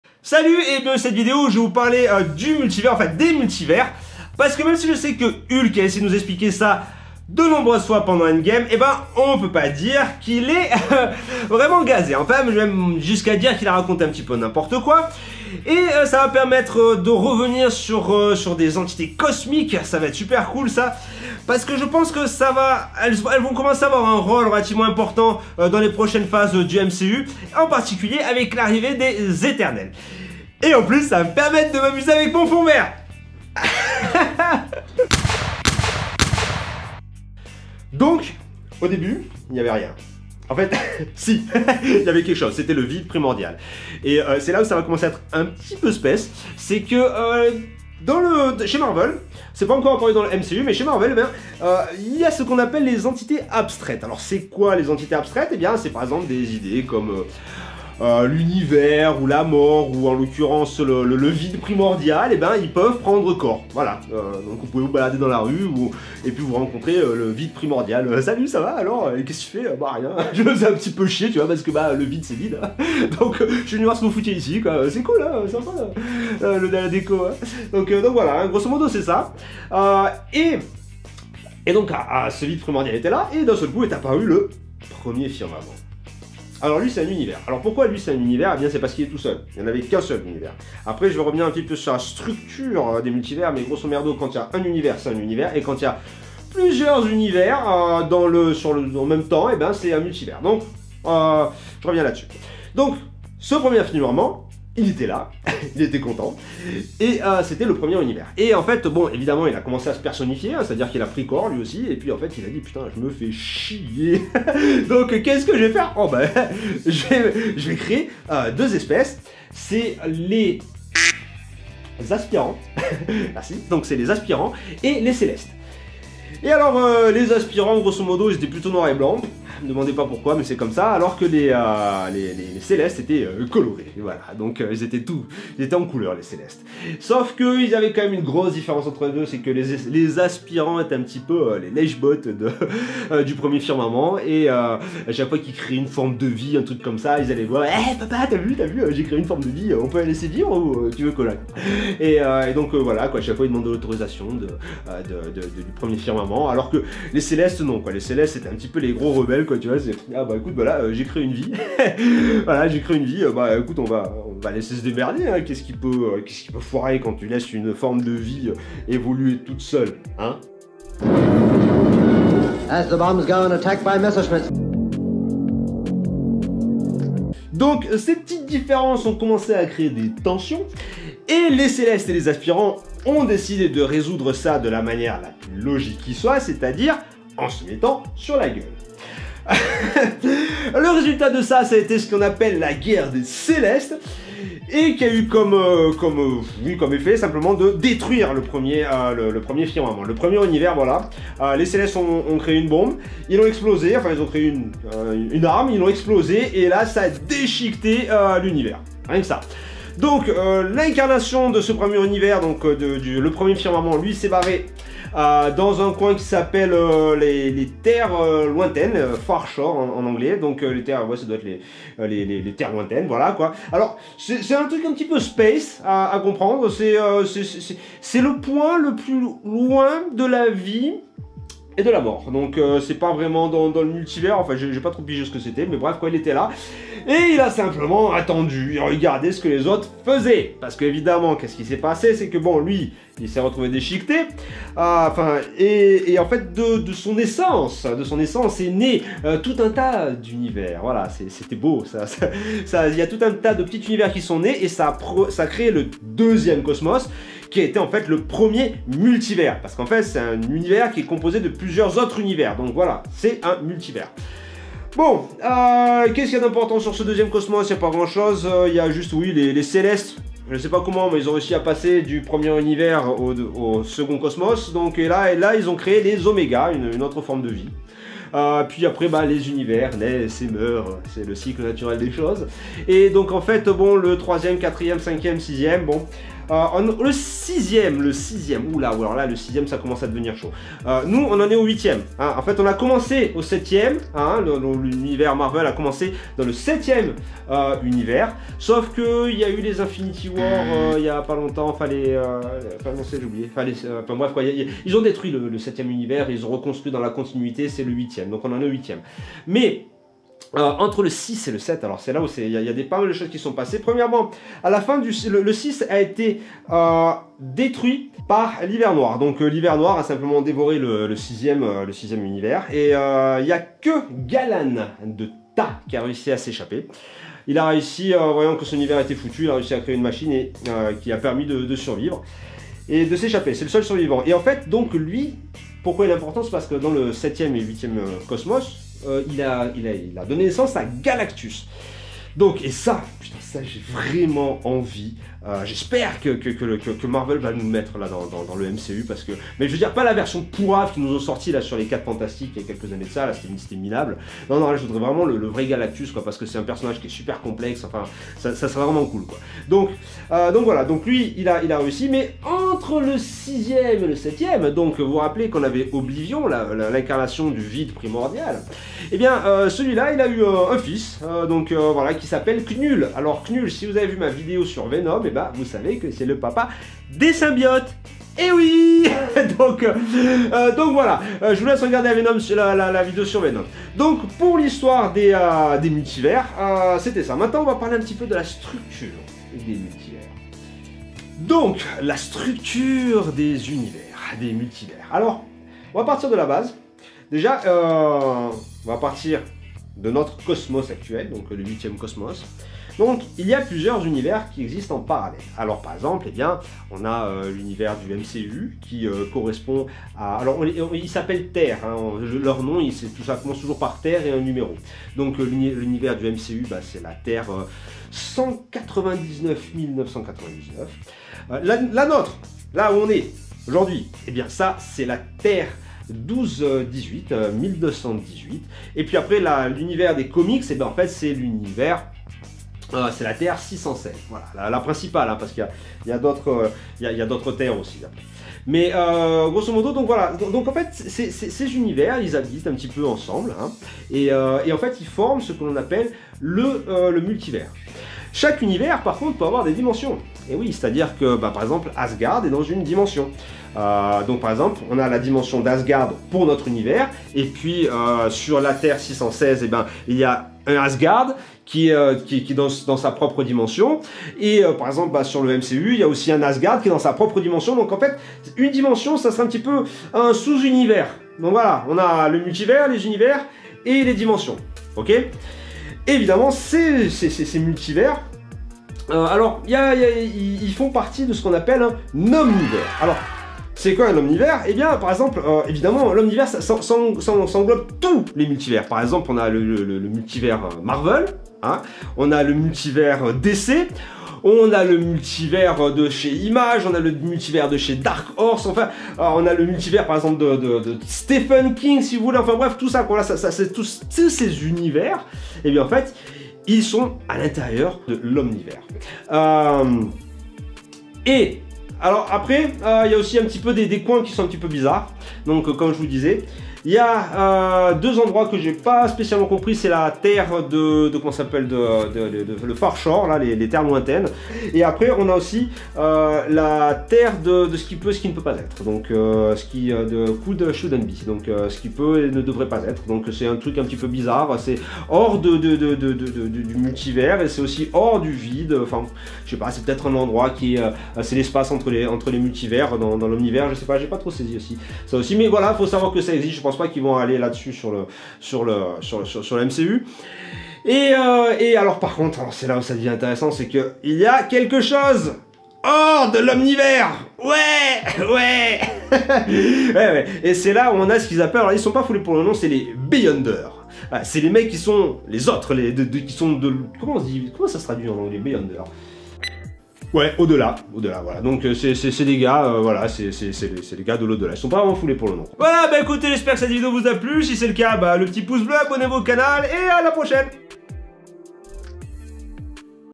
LES MULTIVERS (MARVEL) EXPLIQUÉS
Salut! Dans cette vidéos je vous explique l'hitoire des multivers, depuis sont origine avec le premier firmament et oblivion jusqu'à Eternité.
Je parle aussi brièvement sur la structure des multivers, des dimensions et l'omnivers  sans oublier les places plus ou moins exotiques comme le Far Shore, The land of couldn't be shouldn't be.

Évidemment, comme on ne peu pas parler de tout ça sans parler des Aspirants et de Celestes, j'en touche aussi deux mots.

Amusez-vous bien :)